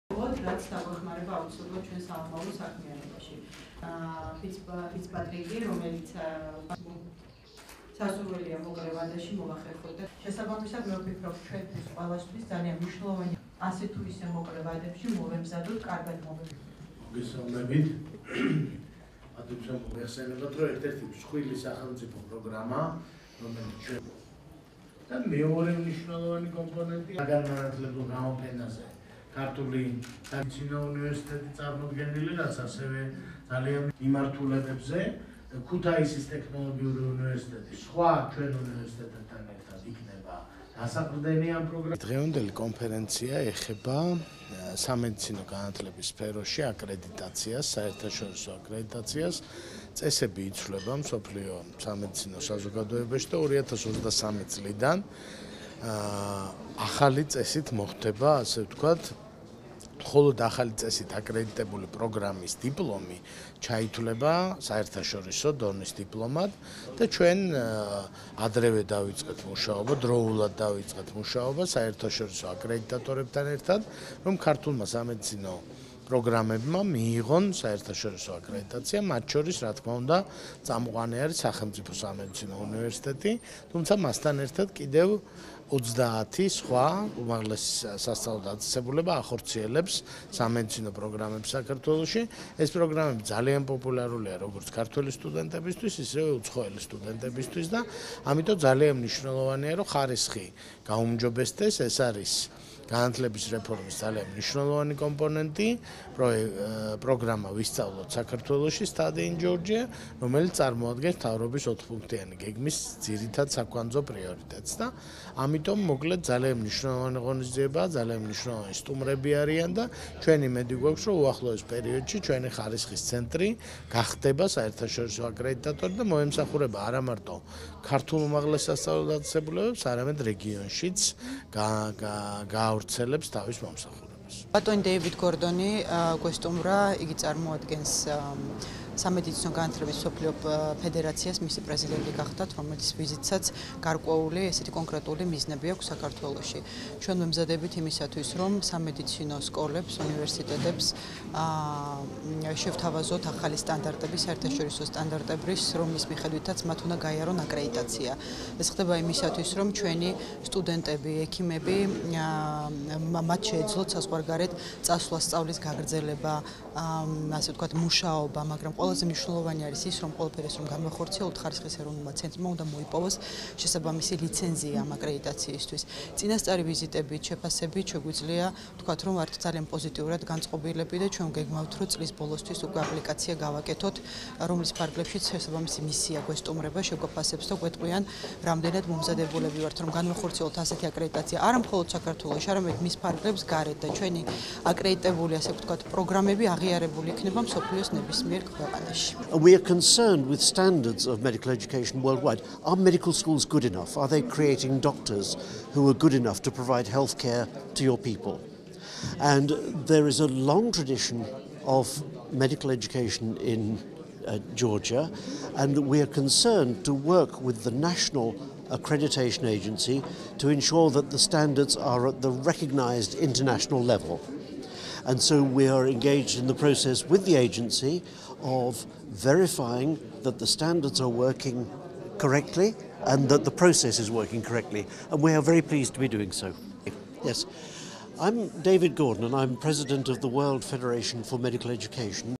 სამედიცინო განათლების მსოფლიო ფედერაციის პრეზიდენტი სამედიცინო უნივერსიტეტს ეწვია
სამედიცინო განათლების მსოფლიო ფედერაციის პრეზიდენტი თბილისის სახელმწიფო სამედიცინო უნივერსიტეტს ეწვია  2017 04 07
President of the World Federation for  Medical Education visited TSMU